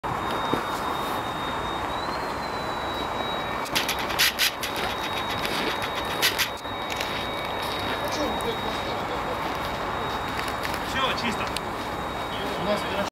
29 января 2012г Осмотр отсека агрегатов Ил-14Т
Осмотр отсека агрегатов на предмет течи топлива. Топливная система находится под давлением.